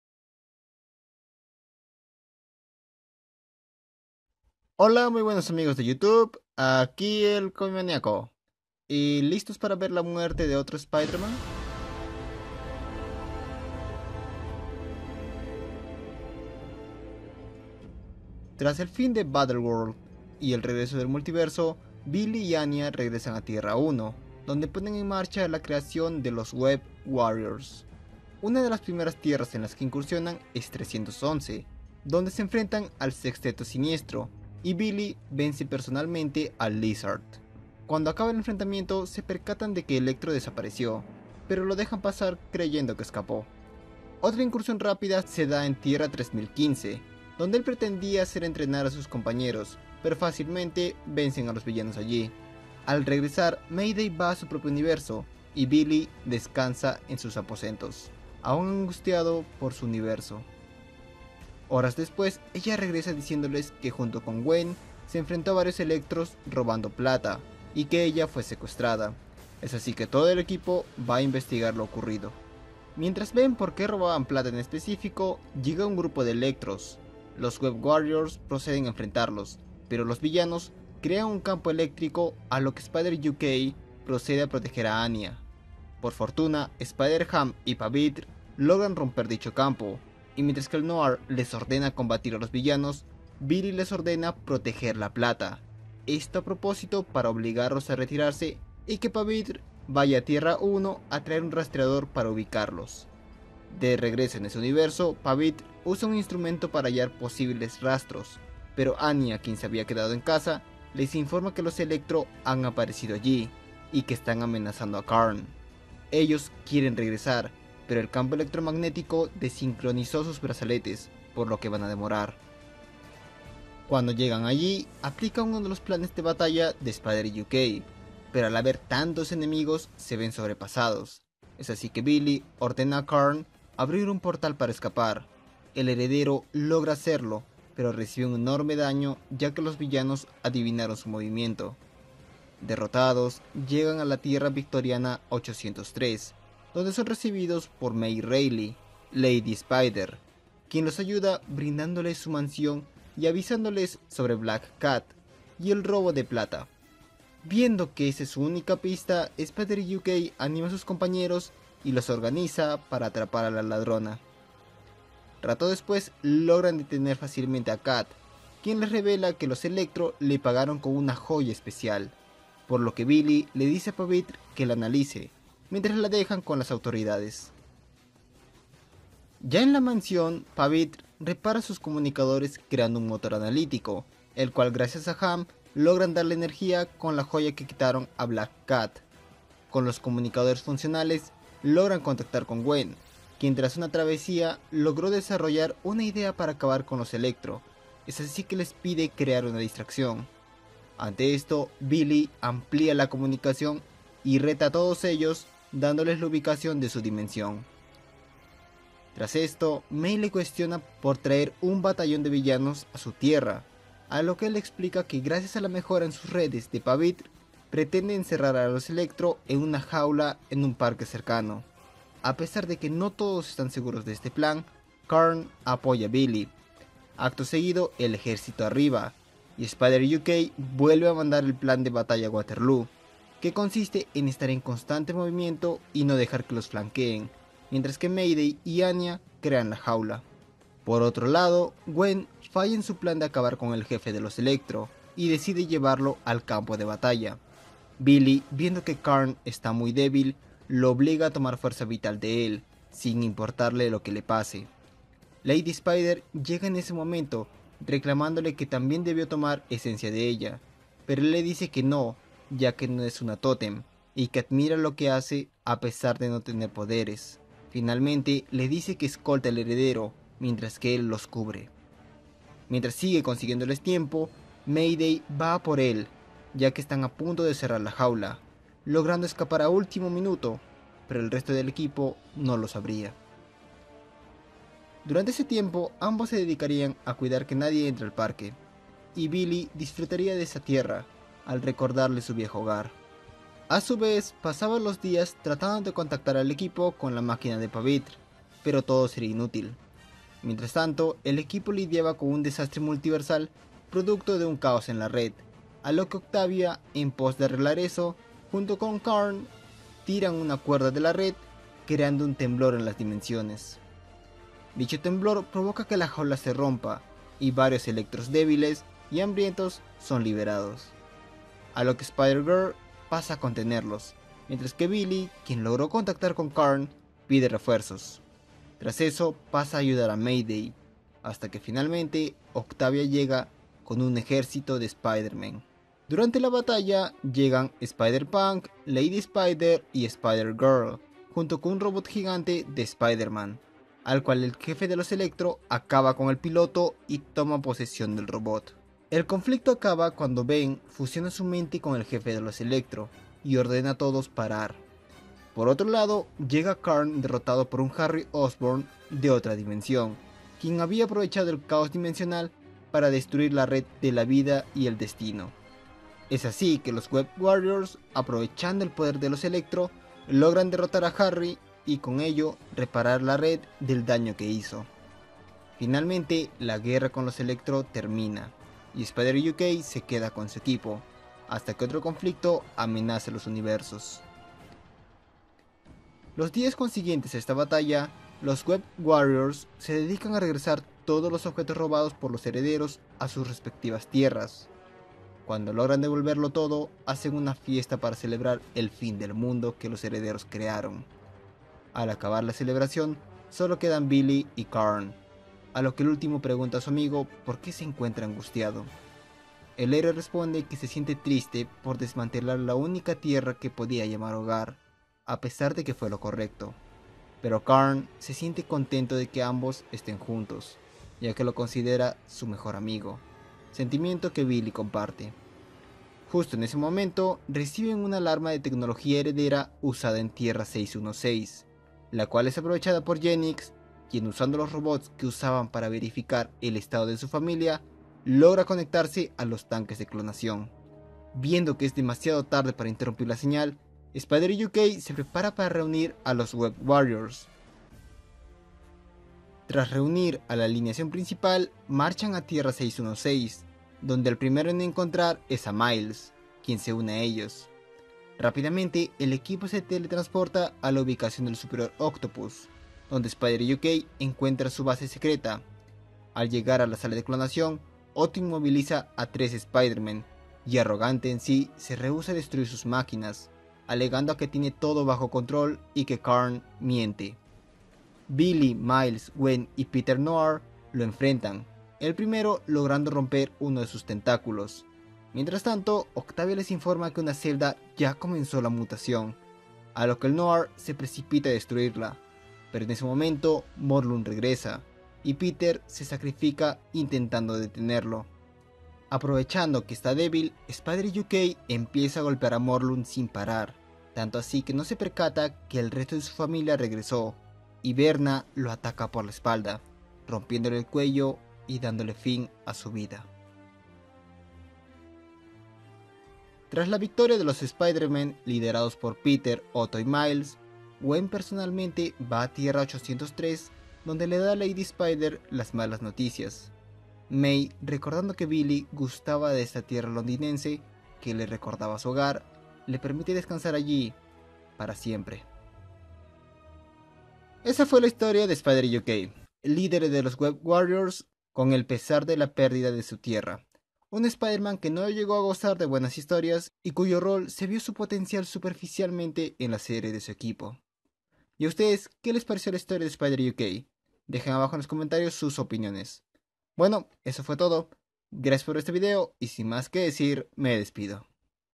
Enciclopedia Spidey | Spider-UK Pt. 2: Electro-Verse, SPIDER-GEDDON y Muerte
"Lo sé. Pero moriré intentándolo, si eso es lo que se necesita para evitar que otro mundo sea destruido."

Hola muy buenas amigos de YouTube, aquí El ComiManiaco, y ya de tiempos que no habia una enciclopedia Spidey, aqui tenemos una con la historia de Spider-UK.

Los cómics de referencia son:
- The Amazing Spider-Man vol. 4 #01
- Web Warriors #01-11
- Superior Octopus #01
- Spider-Geddon #01
- Spider-Gwen: Ghost Spider #04